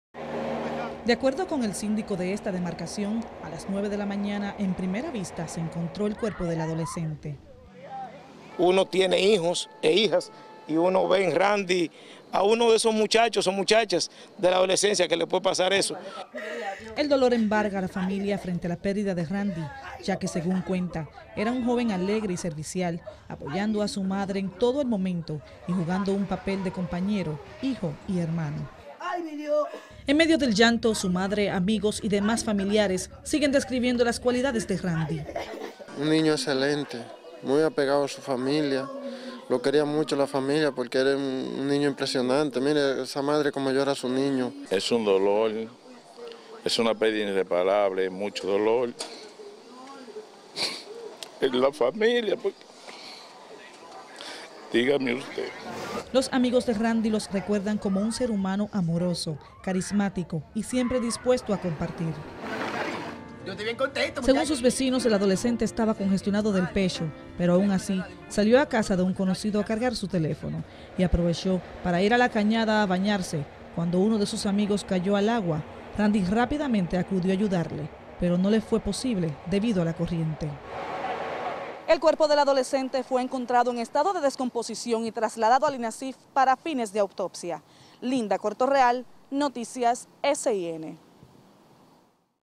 Encuentran cadáver de joven en cañada yaguasa
Luego de dos días de búsqueda, la mañana de este sábado fue encontrado el cuerpo del adolescente Randy Almonte, quien cayó a las aguas de la cañada de yaguasa, en Santo Domingo Norte.